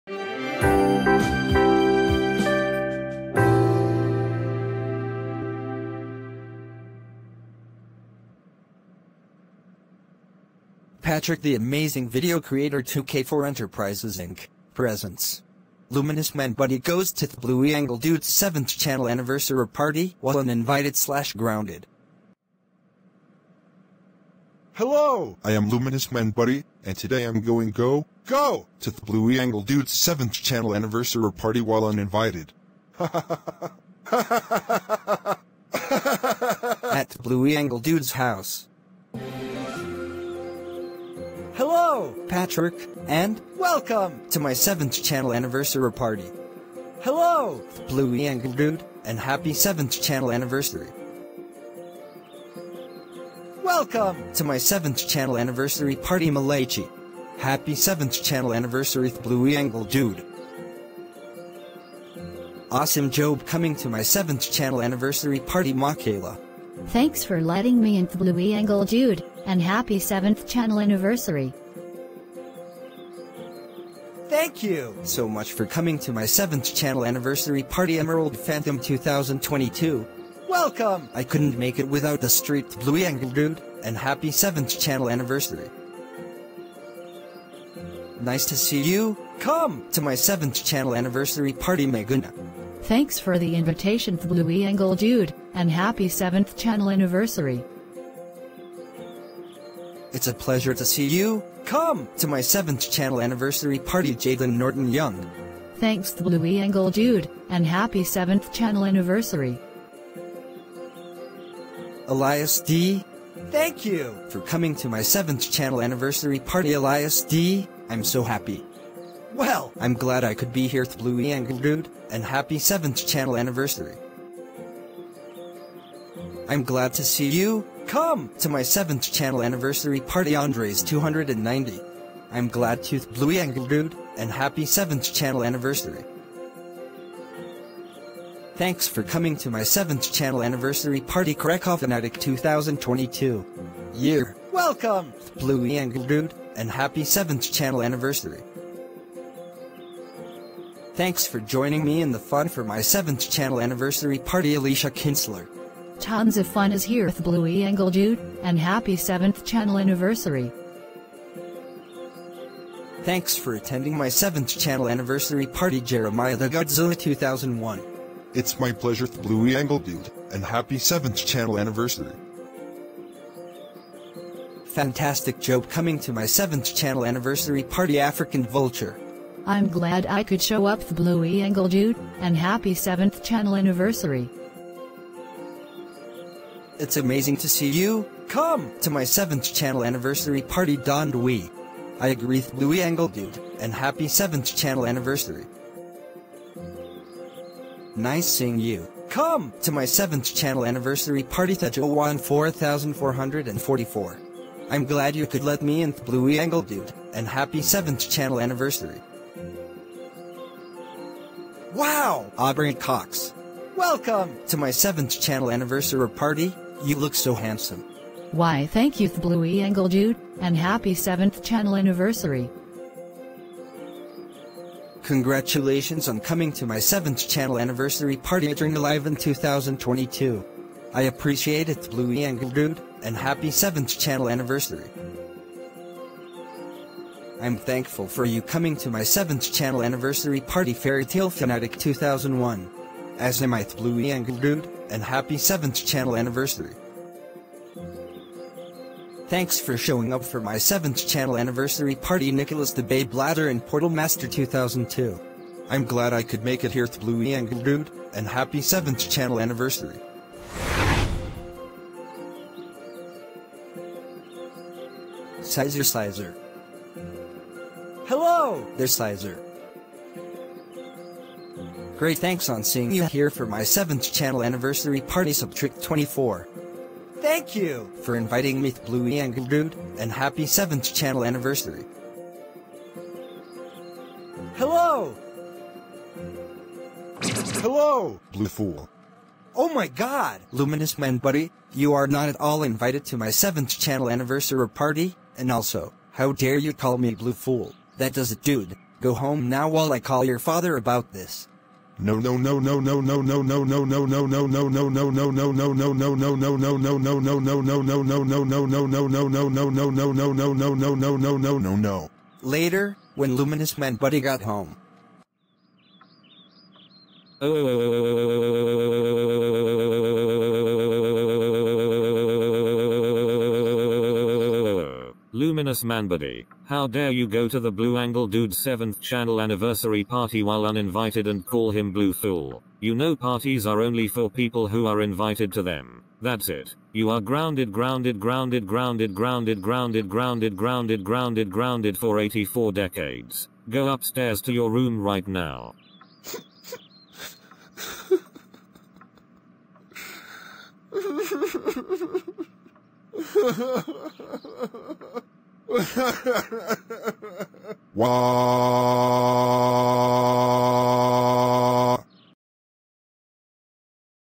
Luminous Manbuddy Goes To TheBlueAngleDude’s 7th Channel Anniversary Party While Uninvited/Grounded
This video is a video special for @AngelRider6’s 7th Channel Anniversary.

Users I added in this video

Me

Malachi

Ma’Kayla

Meguna